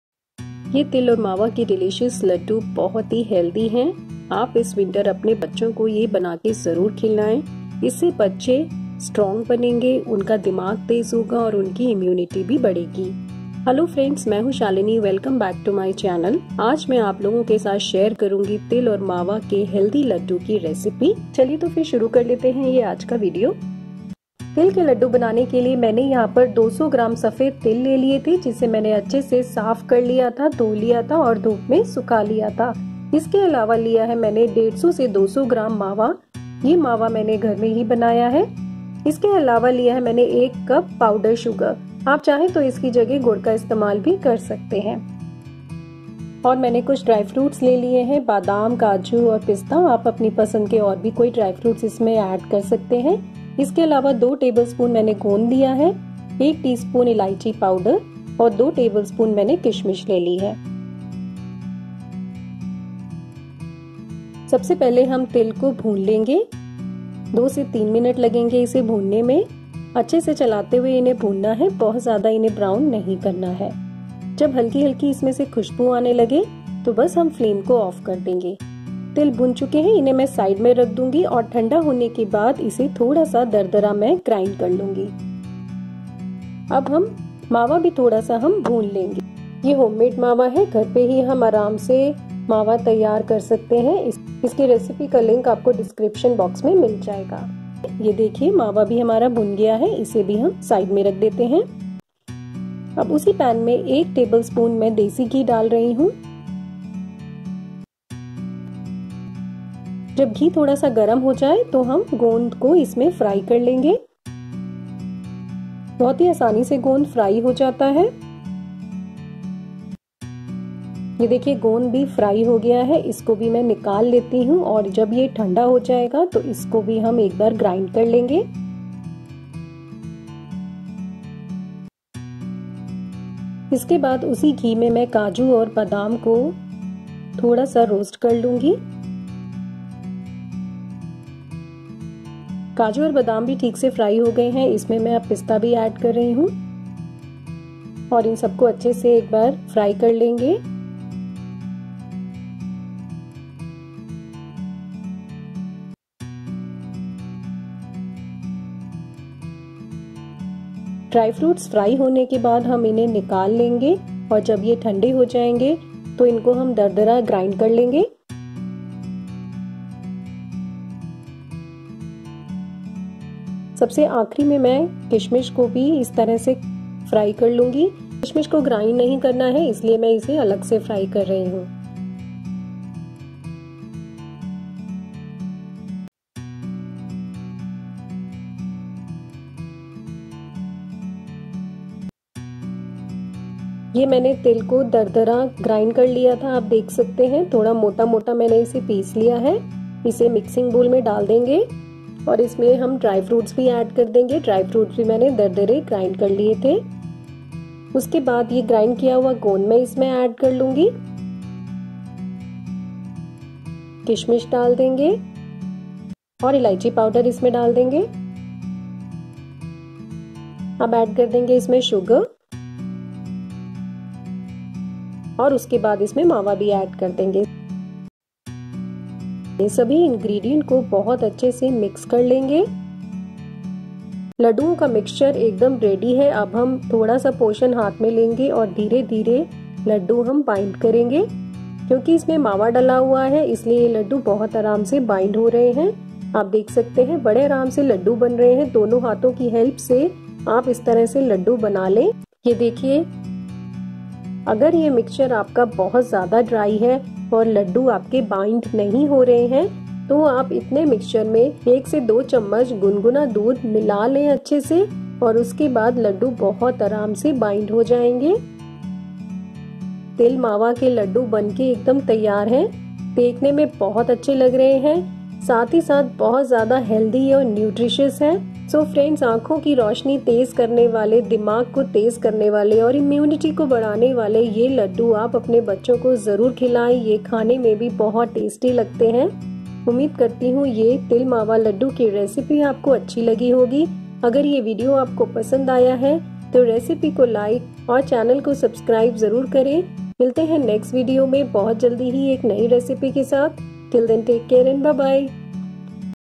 सर्दियों में बच्चों को खिलाएं ये ख़ास तिल और मावा लड्डू | Til Mawa Laddu Recipe- Indian Tadka
Hello friends,

In this video I am sharing quick and easy recipe to make delicious Til mawa ladoo.Til aur mava laddu or Til khoya ladoo is a very common Indian sweet in north India during winters.
So, Enjoy the rich taste of Til Mawa Ladoo this winter season. Relish eating these sweet sugary delights that not only soothe your cravings but are also beneficial for health.

Do give a big like to this recipe in case you liked it, also please do share with maximum friends as possible.